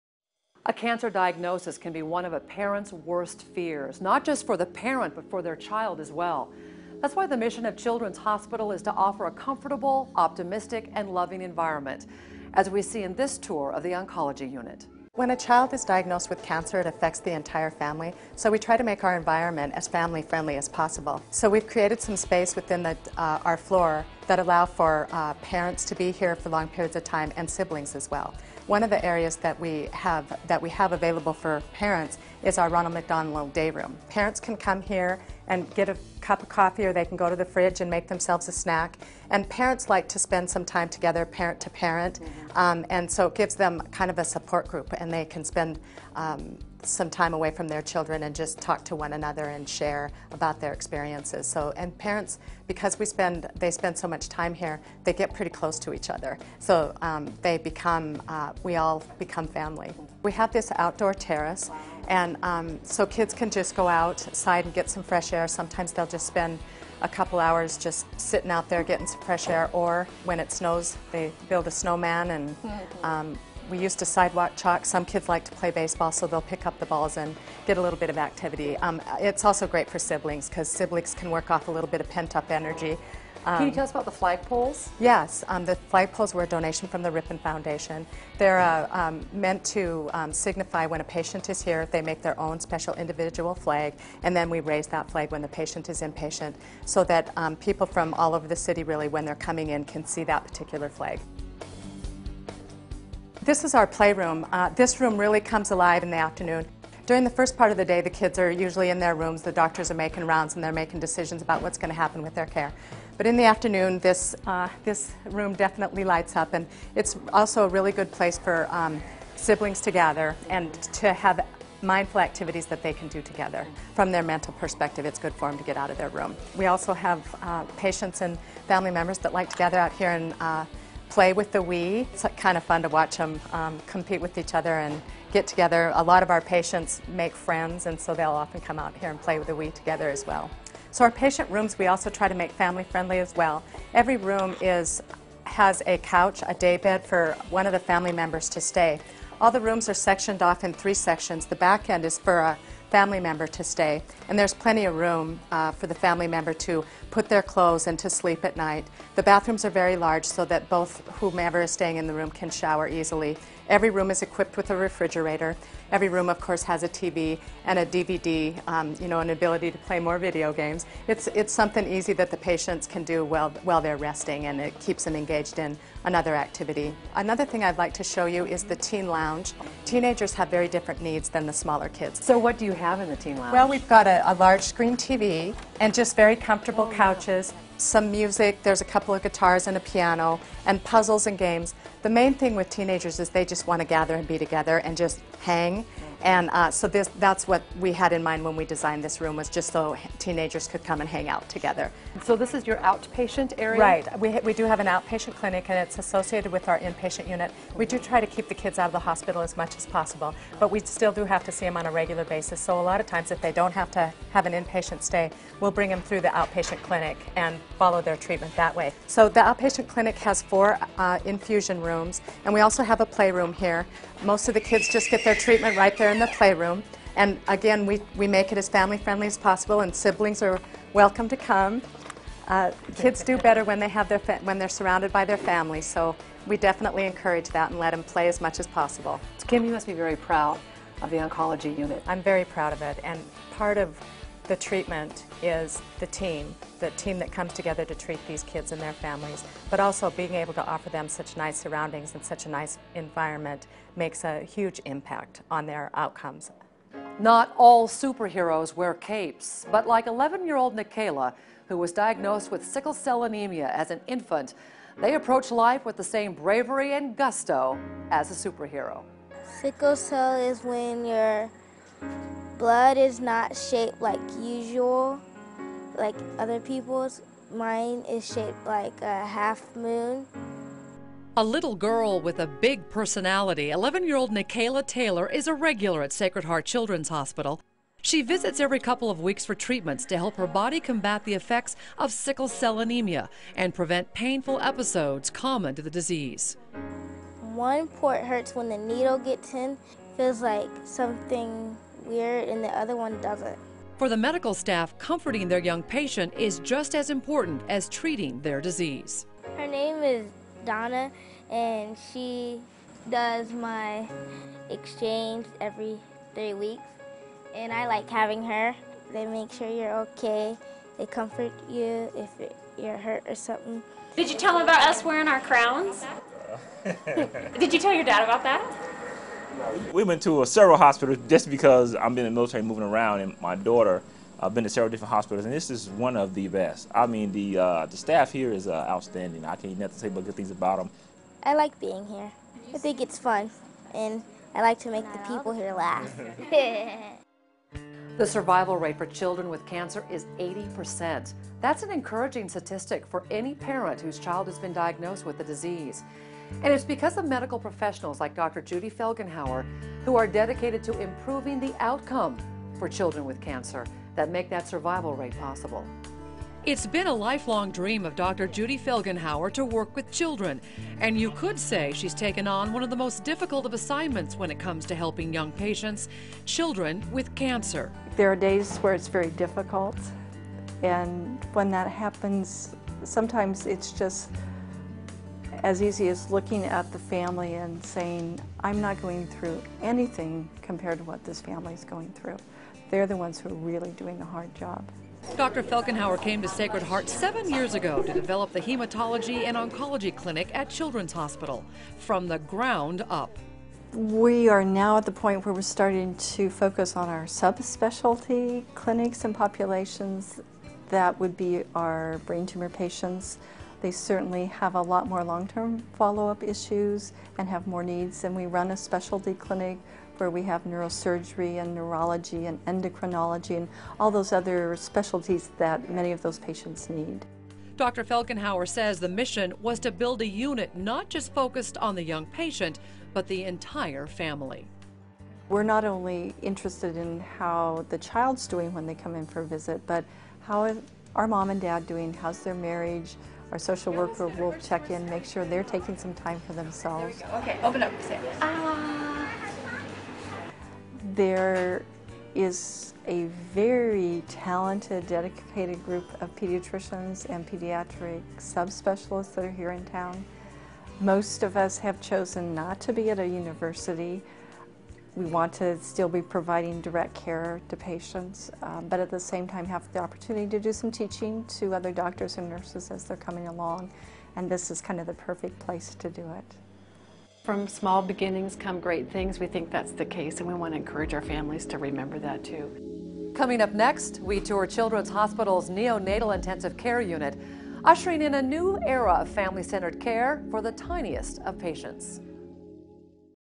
Providence Sacred Heart Children's Hospital Part 2
KXLY4's Nadine Woodward takes viewers on a behind-the-scenes tour of Providence Sacred Heart Children's Hospital in Spokane, WA.